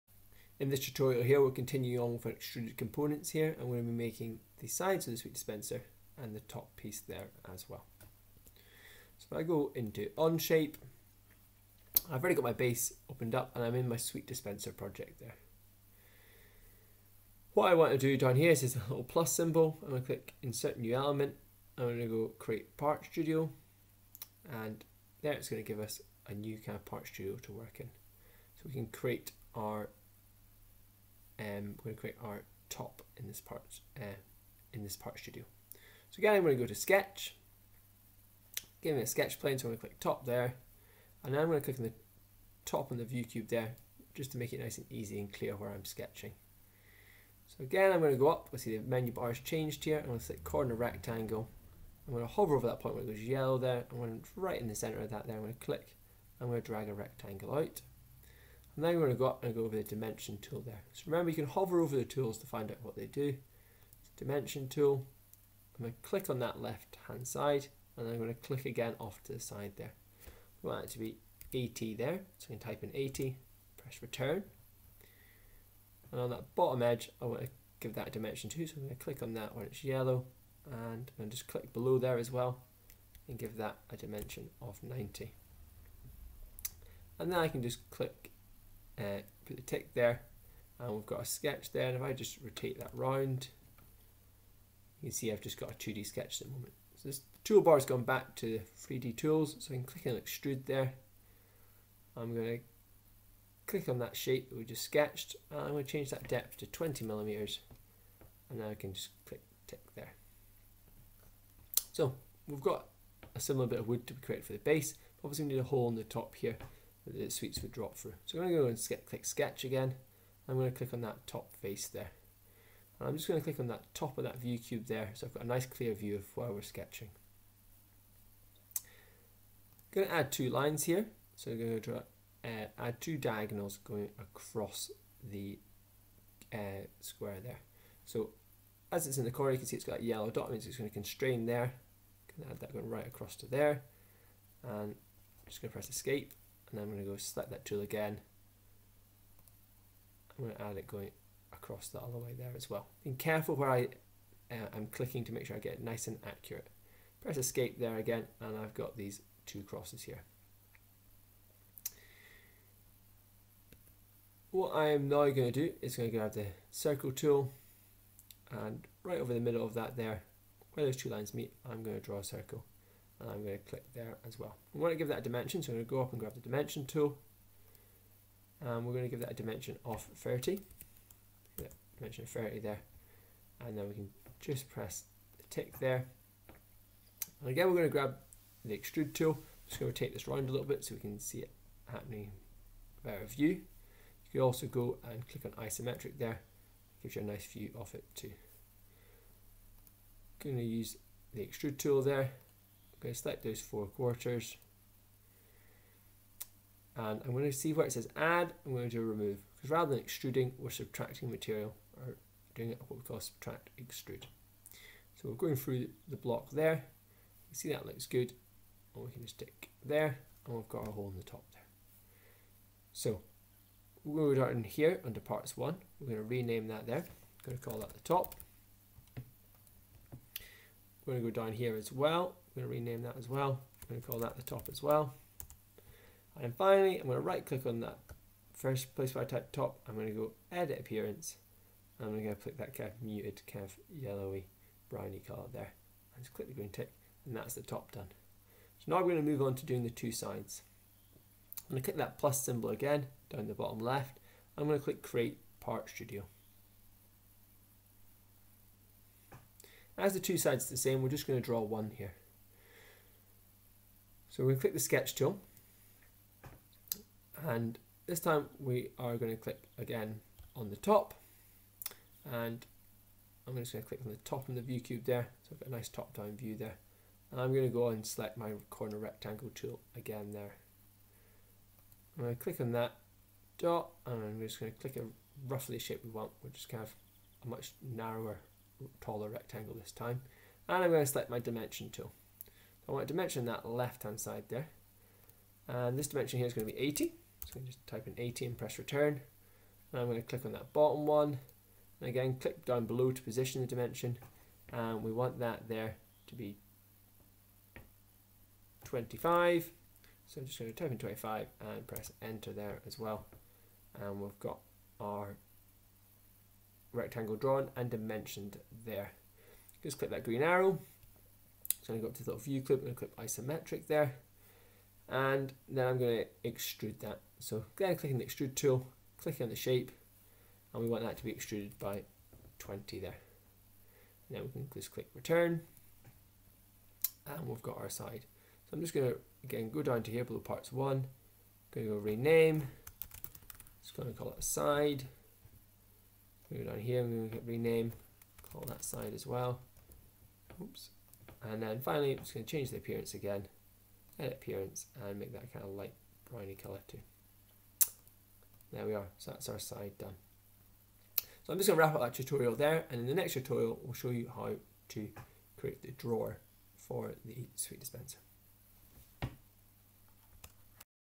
2 - Onshape CAD Tutorial - Sweet Dispenser Top and Sides
2nd CAD tutorial in modelling a sweet dispenser in Onshape CAD software.